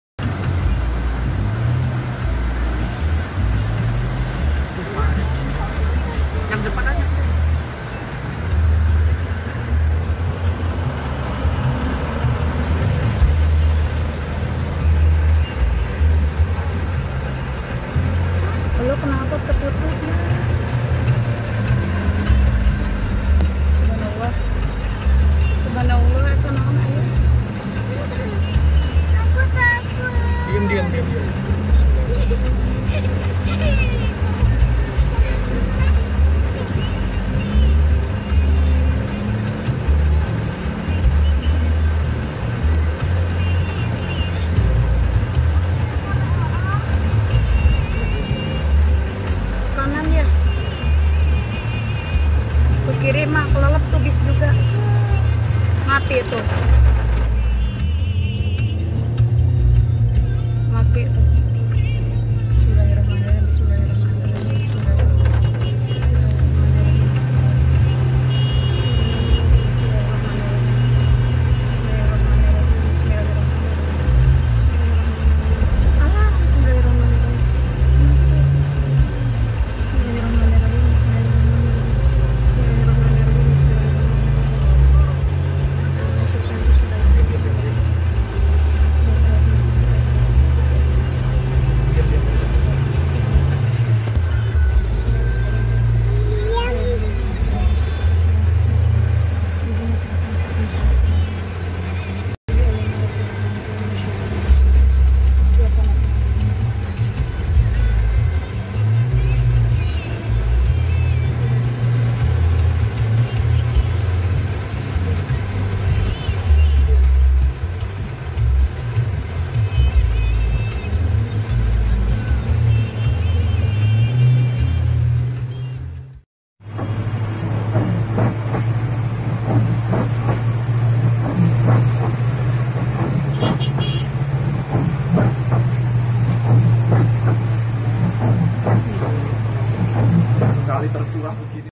banjir tol cipali 31 desember 2019
Banjir Di Tol Cipali 31 Desember 2019

Tol cipali km 138 kondisi sedang banjir dilaporkan jam 17.35 tgl 31 Des 2019 oleh mbak Khasanah .Hati2 bagi para pengendara yg akan kembali ke Jakarta dan Bekasi
Semoga mbak Khasanah dan keluarga sampai di tujuan dengan selamat dan terhindar dari bencana .

aamiin

Bagi yang mau melalui Tol cipali atau Tol manapun yang ada kecenderungan banjir, hati hati ya